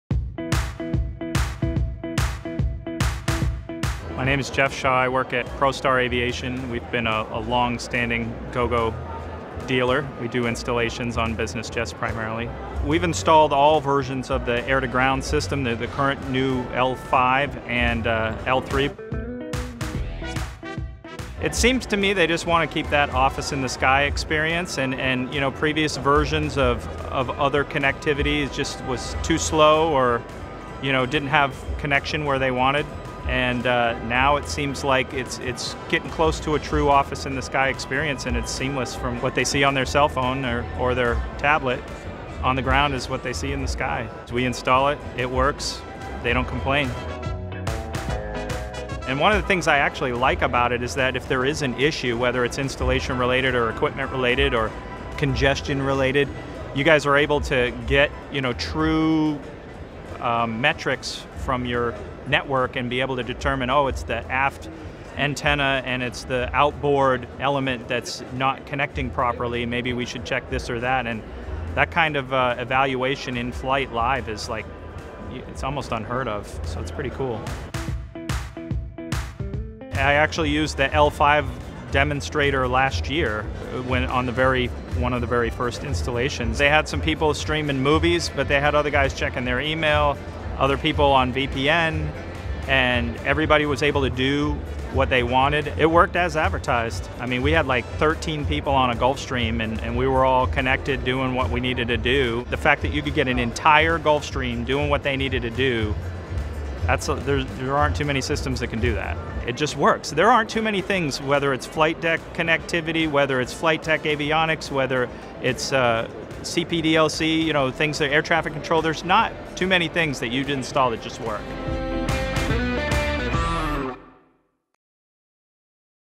Pro Star Aviation delivers a true "office in the sky" with Gogo AVANCE L5
Hear Jeff Shaw from Pro Star Aviation explain the performance of AVANCE L5 and why he recommends it to his customers. He raves of the Gogo AVANCE L5 capabilities from video streaming and VPN. To learn more about Gogo Business Aviation and the AVANCE L5,